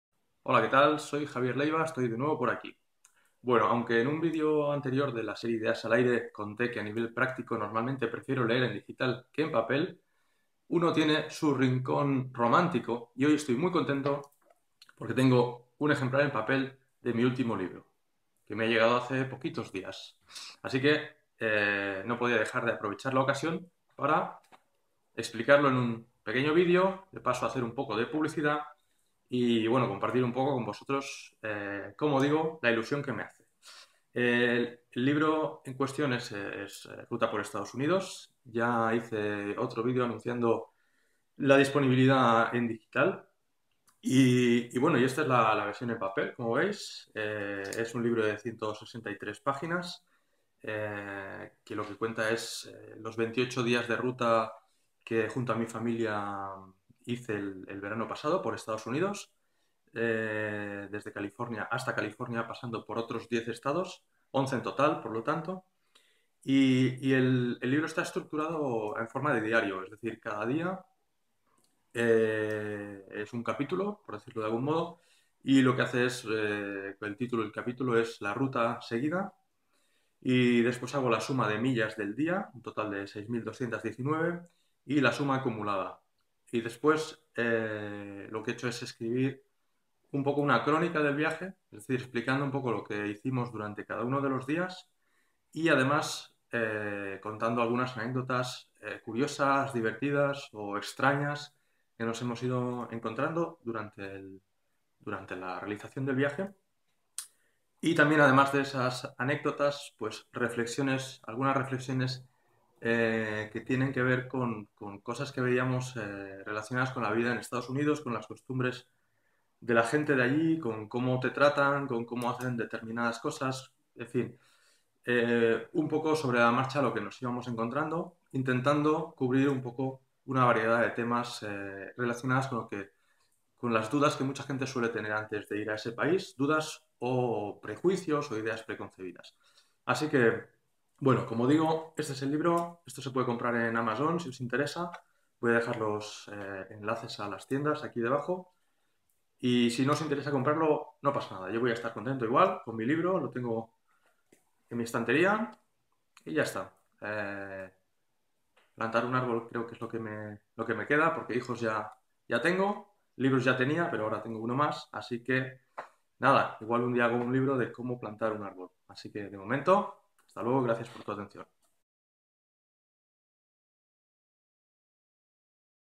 Ruta por Estados Unidos: 6219 millas por la Costa Oeste (Road Trip) [libro]
En este libro presento la versión en papel de mi último libro: Ruta por Estados Unidos